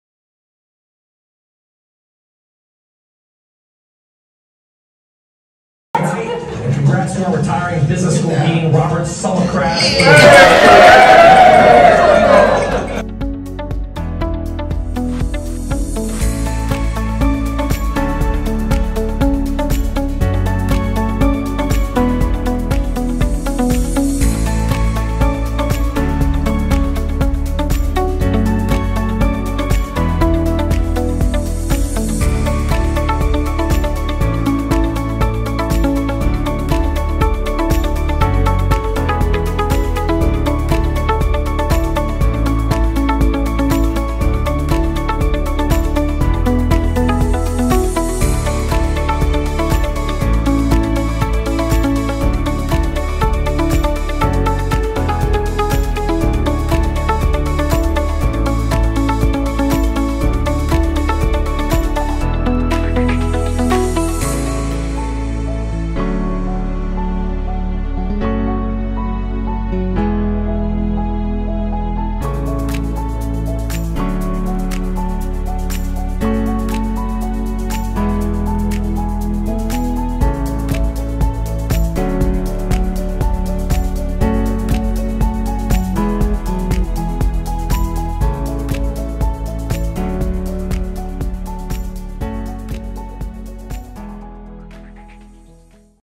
Hokies on Wall Street 2022
The 20th celebration of Hokies on Wall Street. In Commemoration of 100 Years of Women at Virginia Tech, there was a short panel discussing current trends in the finance industry and networking featuring women leaders on Wall Street followed by a networking event.